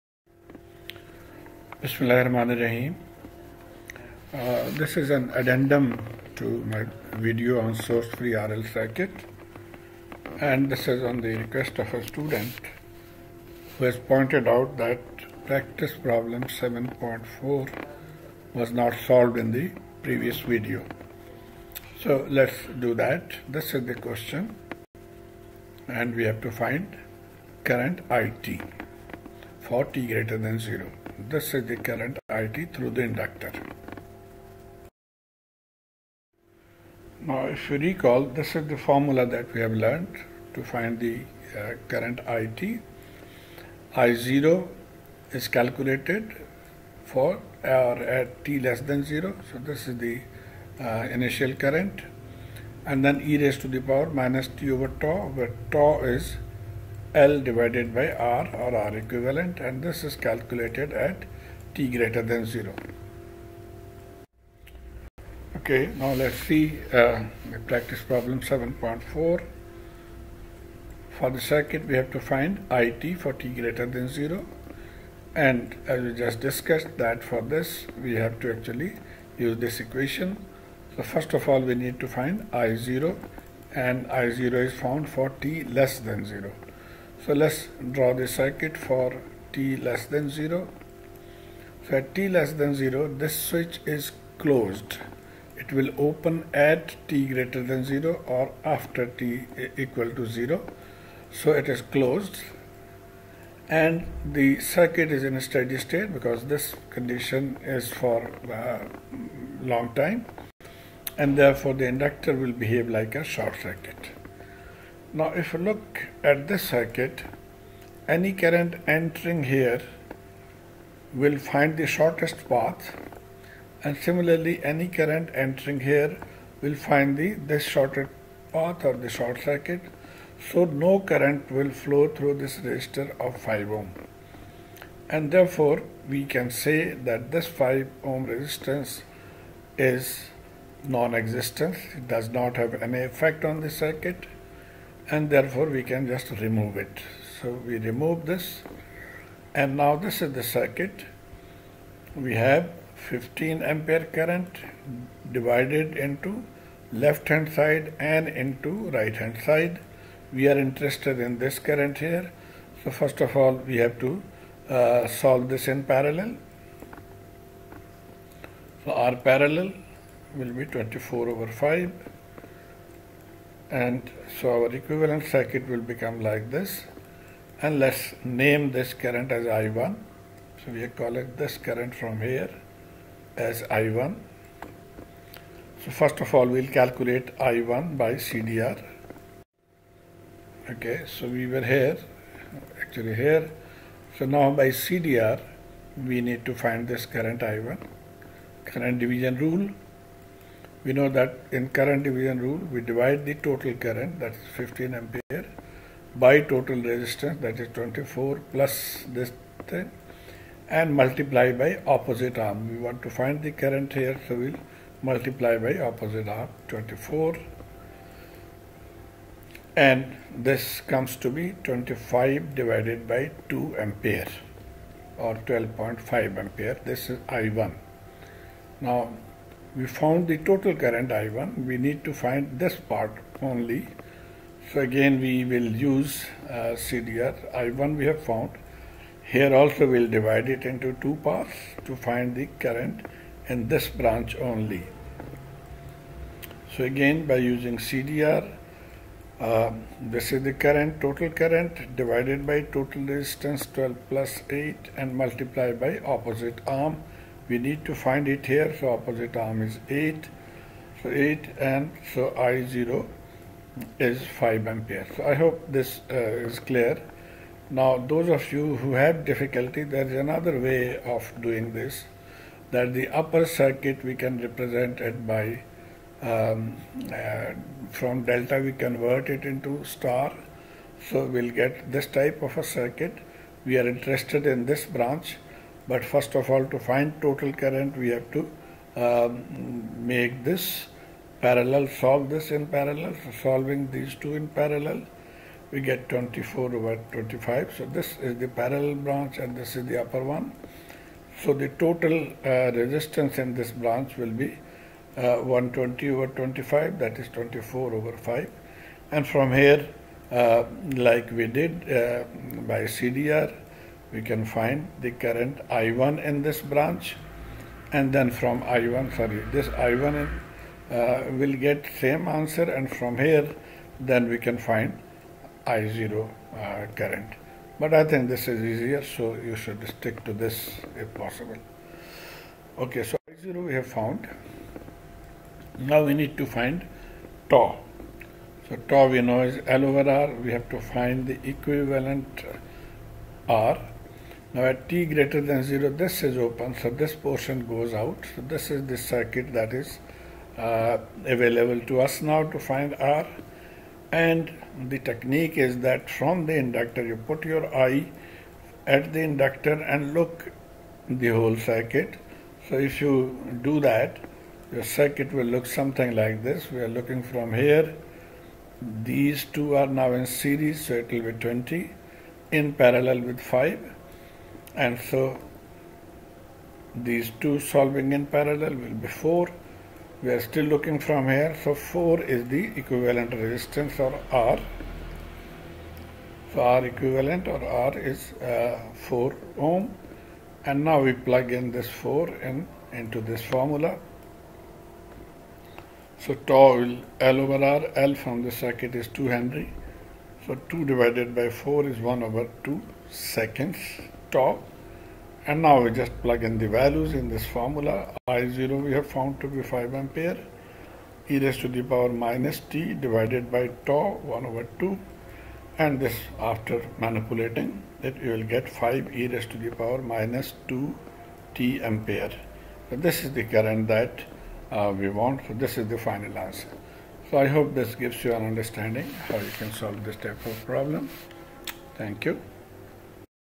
(English )LCA Practice 7.4 || Source Free RL Circuit
Practice Problem  7. 4 (English) Source Free RL Circuit (Alexander & Sadiku) -

In this video, we solve Practice Problem 7. 4. Source Free RL Circuit

Practice Problem 7.4: For the circuit in Fig. 7.18,  find i(t) for t greater than 0.

The series connection of a resistor and an inductor
Our goal is to determine the circuit response,